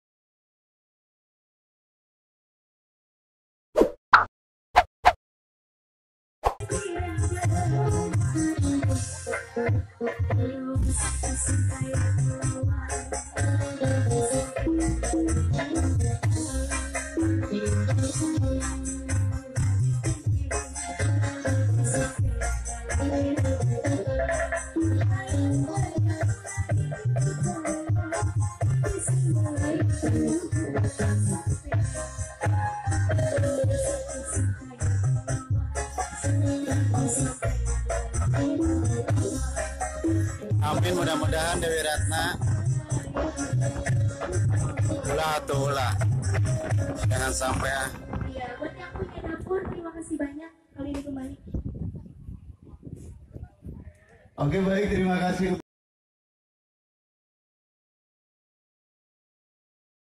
Viral Video Penyanyi Dangdut Konser di Depan Orang Mati
Pernikahan ini digelar di atas tanah kuburan

Tak hanya tenda yang terpasang, tapi juga sebuah panggung dangdutan